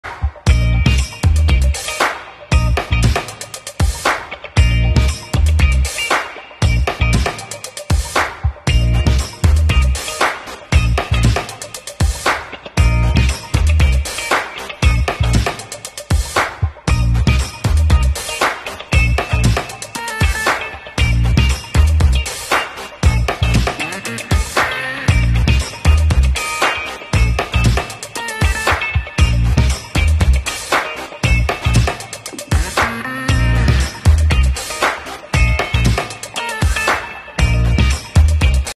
Chocolate Royal Icing Cookie Painting
Painting on chocolate royal icing or any edible canvas light or dark is easy with Sugarprism Edible Acrylic Paint because Sugarprism is super opaque just like real acrylic art paint medium. You can also airbrush with it, stamp , palette knife, screen print and so much more and its a great vanilla flavor that wont ruin the taste of your treats!